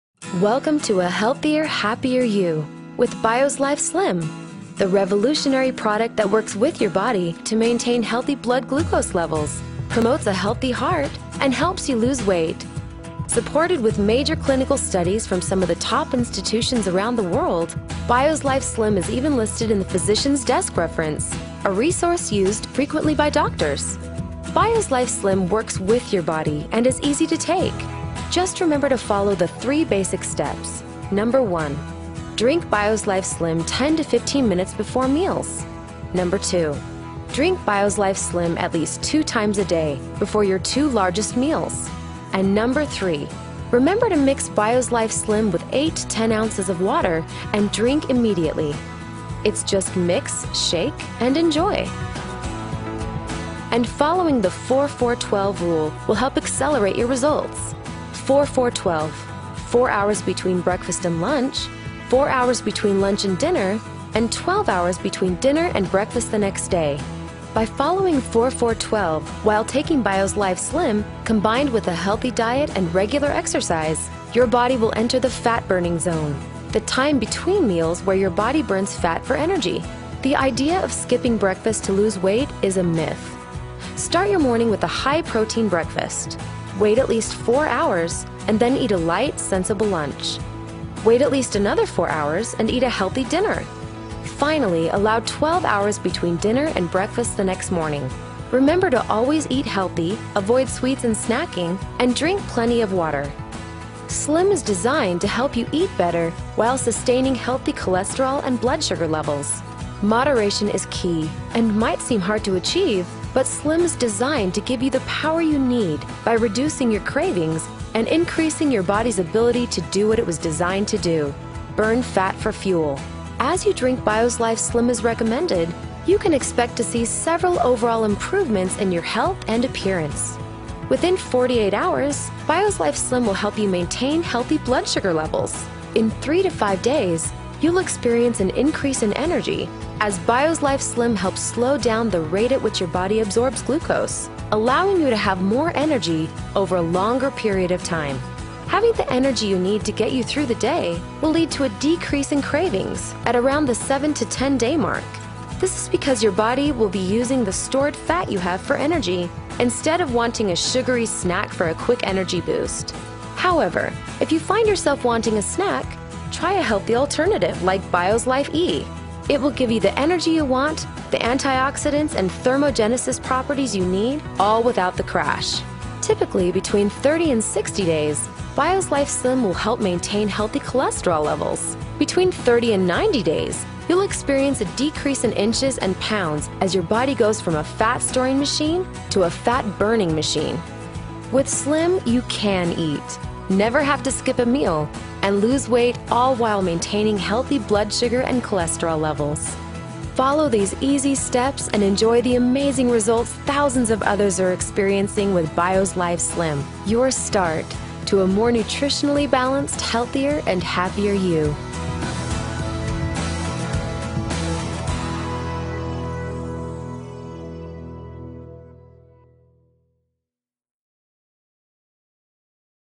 How to Take Bios Life Slim_Chinese Subtitles
Bios Life Slim helps in two ways. One, Bios Life Slim will slow the rate of absorption; this is especially significant for high-carbohydrate meals. Carbs are great sources of energy, as long as they're used relatively quickly. But if they're not used, these carbs are converted into fat. By slowing the absorption rate, Bios Life Slim helps your body utilize those carbs over a longer period of time - eliminating the need to store them as fat.

The other way Bios Life Slim helps is by creating fat-burning zones between meals. Utilizing the 4-4-12 principle (where you wait at least four hours between breakfast, lunch, and dinner, and 12 hours between dinner and breakfast the next day) it allows your body's glucose and insulin levels to return to normal. You won't have to eat between meals because the fiber in Bios Life Slim will be providing ongoing energy form the carbs you've already eaten, and if you do need more energy your body will have a reason to burn the fat reserves it already has.

In the end, Bios Life Slim not only helps curb hunger, but it helps your body burn the fat it already has - all without the confusion, hunger, and mediocre results of other weight loss products and programs.